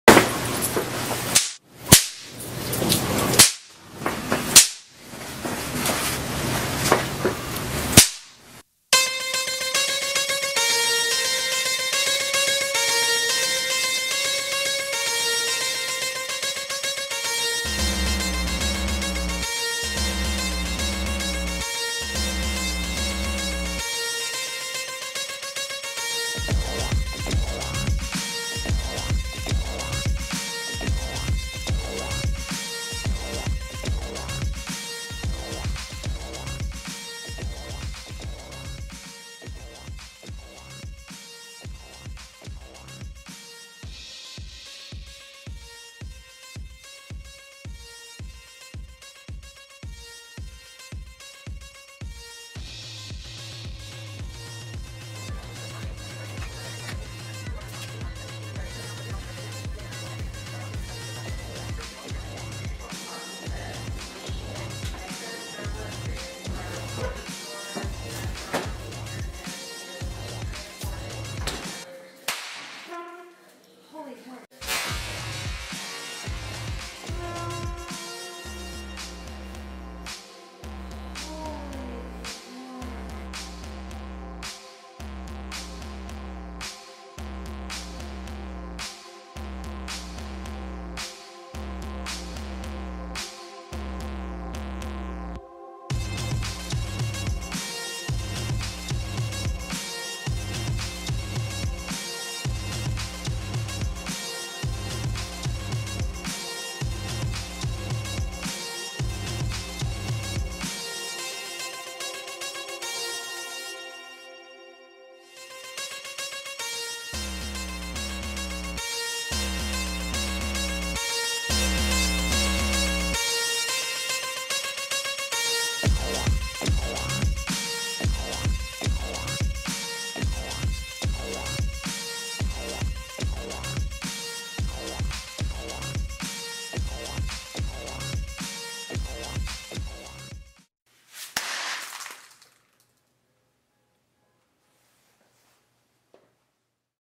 DIY Super-Mega Party Popper/Snapper
Take your next party and fun to the next level with this simple DIY party hack!  I did and it was as safe as you might expect!